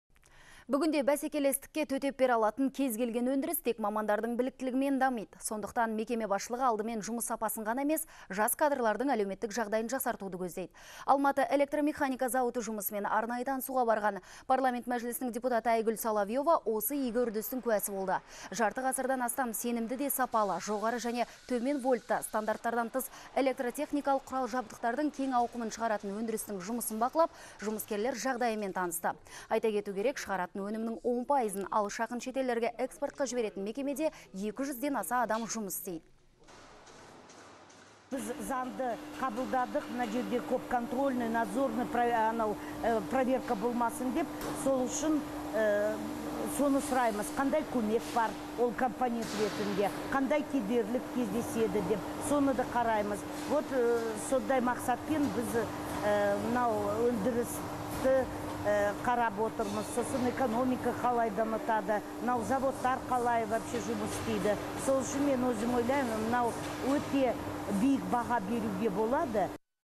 Маман жағдайы түзелмей, өндіріс өркендемейді
Бүгінде бәсекелестікке төтеп бере алатын кез-келген өндіріс тек мамандардың біліктілігімен дамиды.